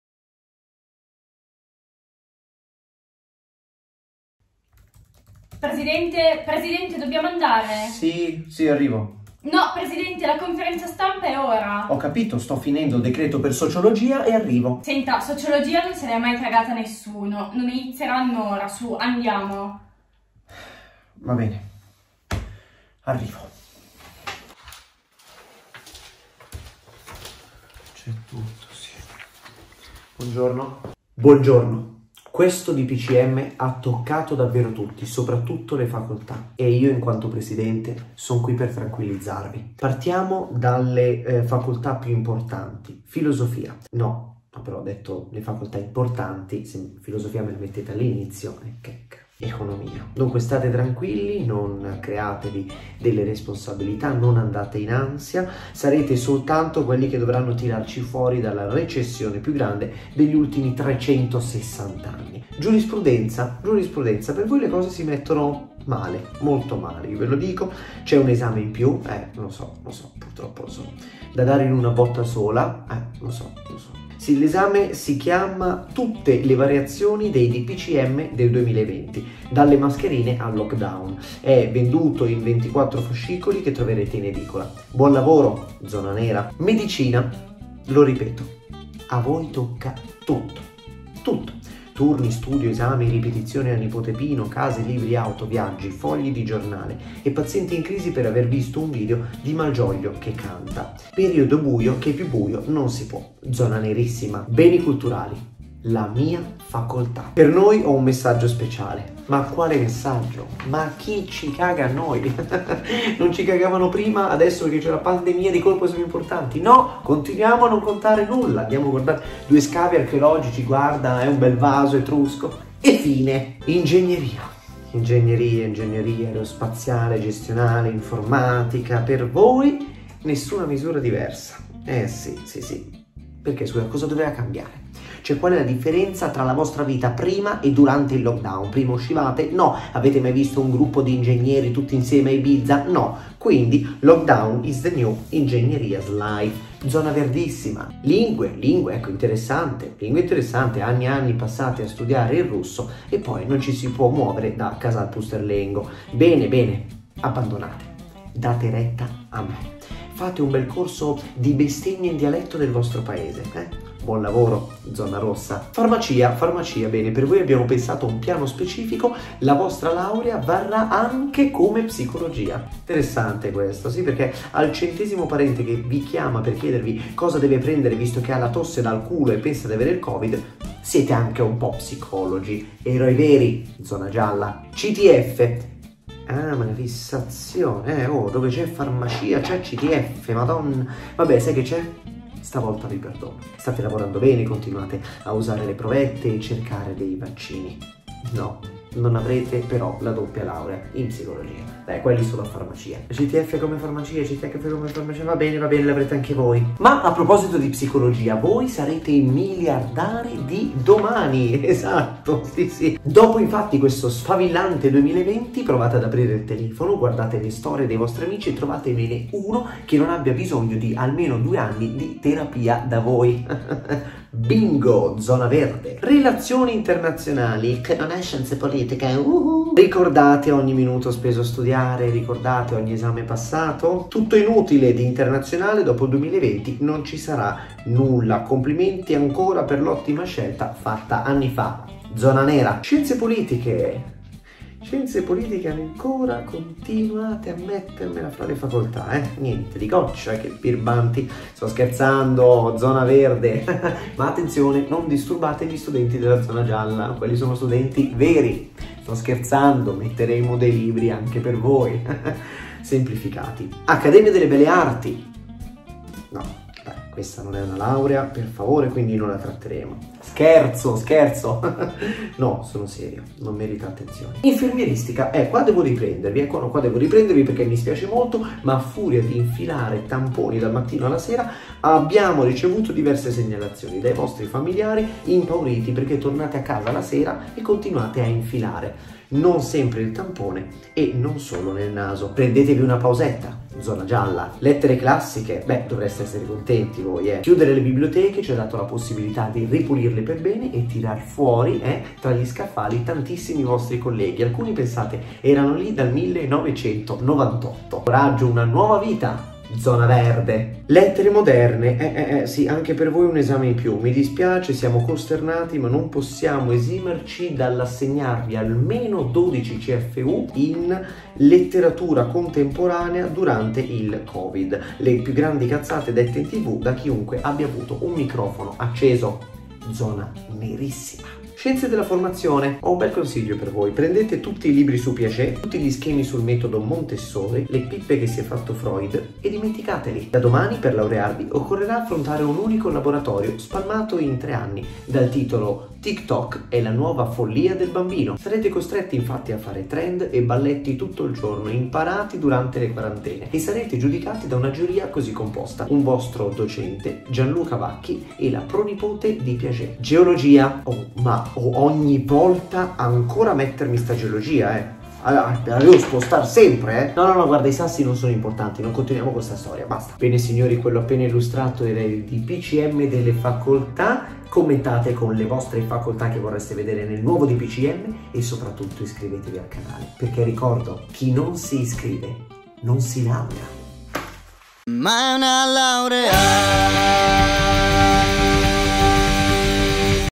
Le FACOLTÀ nei DPCM
Il mondo delle facoltà è stato ignorato dai vari DPCM, allora ci ha pensato il Presidente degli universitari a parlare della nostra vita in questo periodo.

Facebook lo usano soltanto i vecchi, ma se ti piace il vintage io faccio cose qui 👉🏻